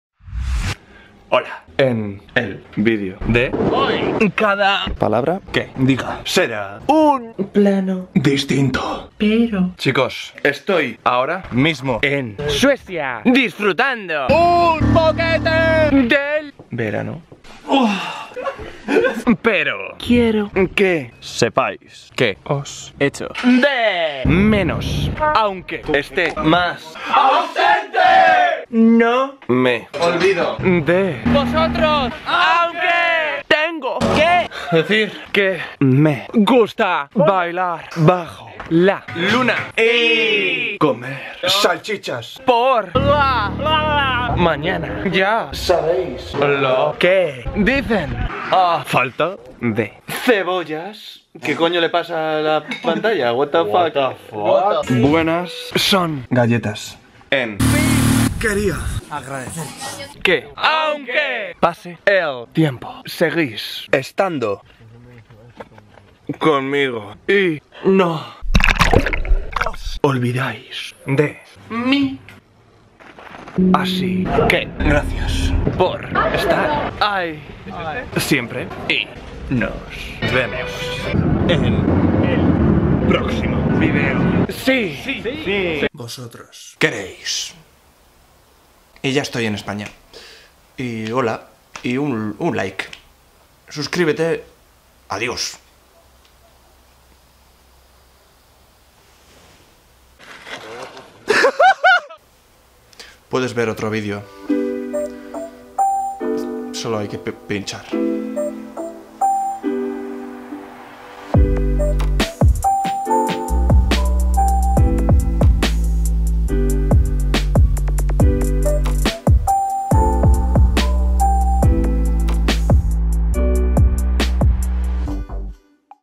1 PALABRA 1 PLANO DISTINTO
Hoy os traigo un vídeo grabado palabra por palabra con planos diferentes mientras disfrutaba del verano en Suecia! Espero que os guste ^^
Mis redes :)

- ¿QUIERES ENVIARME UN REGALO O CARTA? :)

28707 San Sebastian de los reyes (Madrid)